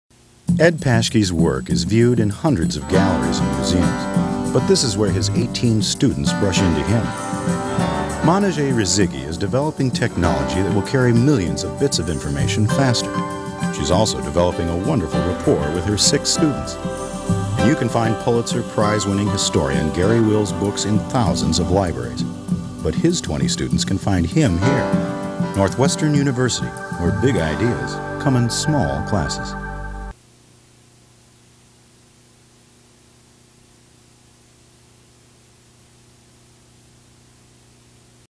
Northwestern University, Where Big Ideas Soar (Version #2)
Northwestern University Promotional Spot from 1996 featuring Prof. Manijeh Razeghi